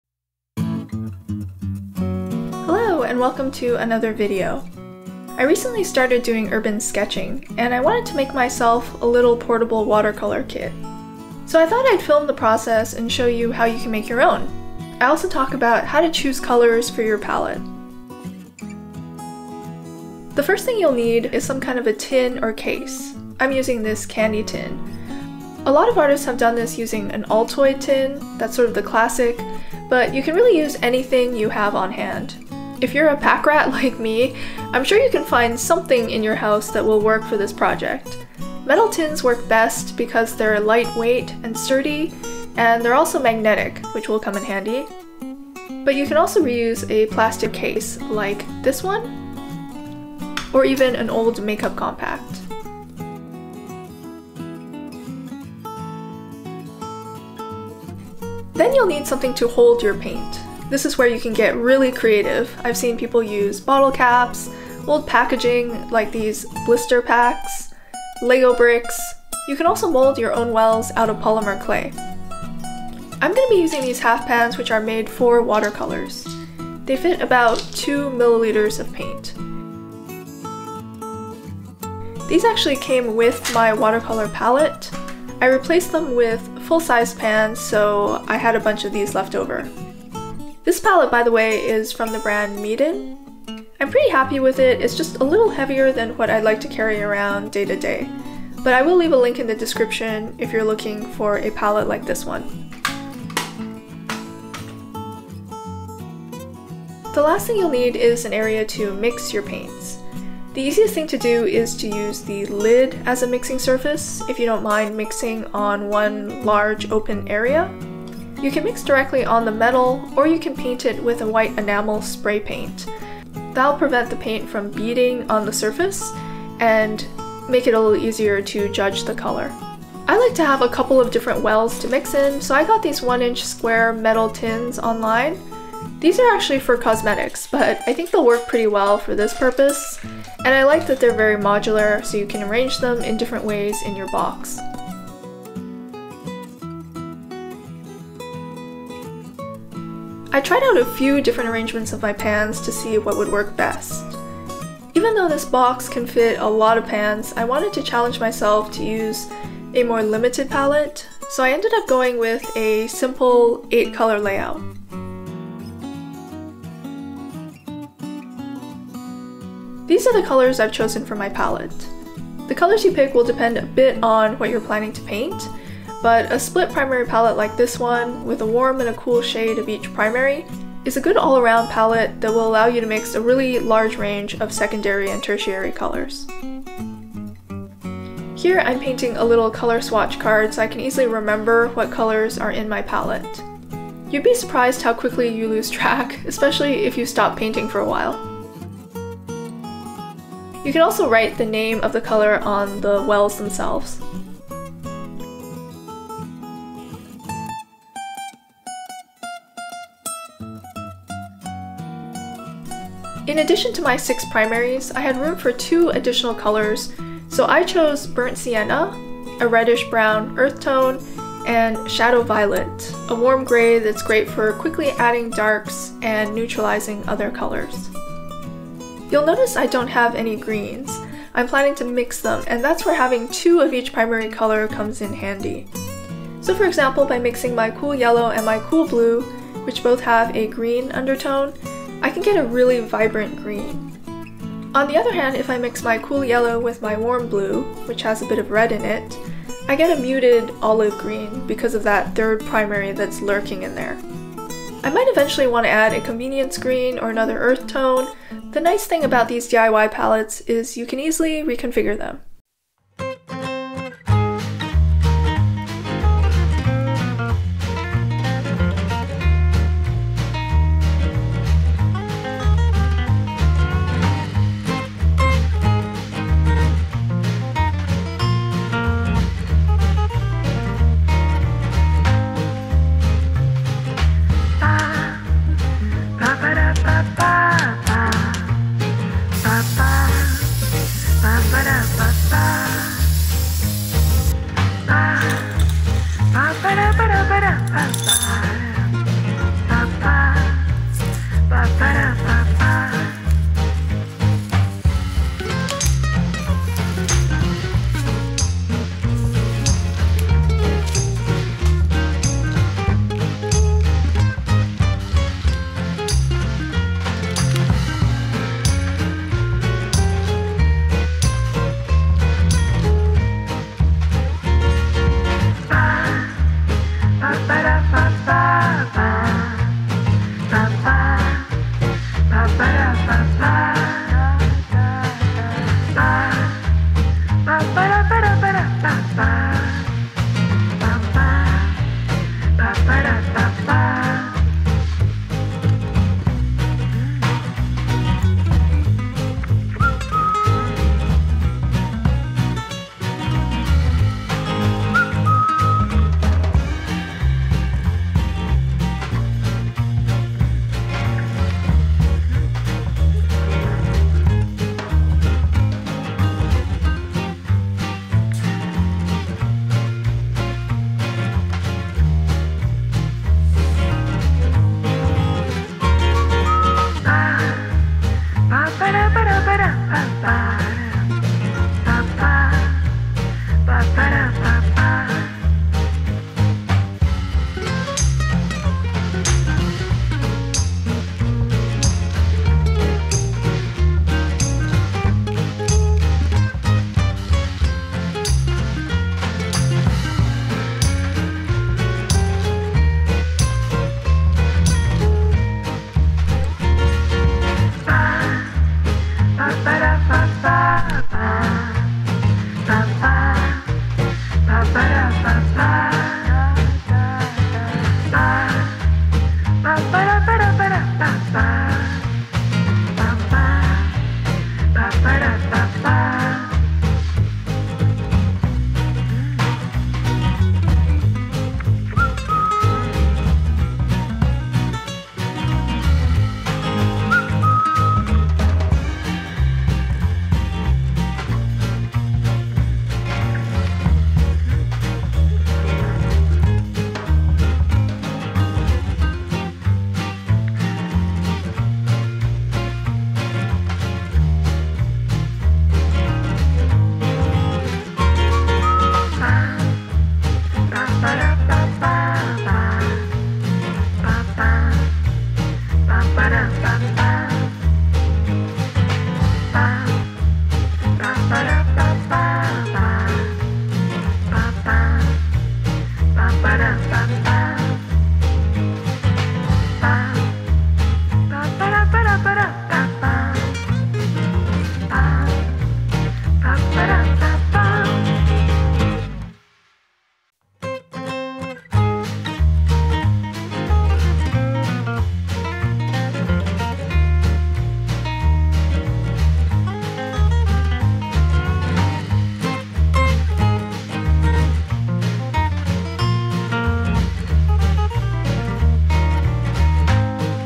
How to make a DIY travel watercolor palette
I made myself a simple, travel watercolor kit using an old candy tin. It's a neat idea that's been around for a while, I think I first saw it

If you have an old tin lying around, definitely give this one a try! It's a fun project and it's nice to be able to paint almost anywhere with this portable, DIY kit.

(And no, this is not an extended ad for Fujiya candy 😊)

✹ Products Mentioned ✹

MEEDEN Watercolor Palette
Square Metal Pans (26mm)
Rust-Oleum Appliance Epoxy Spray Paint
Pentel Aquash Water Brush
*Pro tip: You can use a little cork stopper from the hardware store to turn a regular brush into a collapsible travel brush.

Chapters:
0:00 Intro
0:20 Choosing a tin or container
0:58 Paint wells
1:47 Mixing area
2:29 Arranging your pans
2:49 Choosing colors
4:25 Assembling the kit
5:56 Sketching trip
8:10 End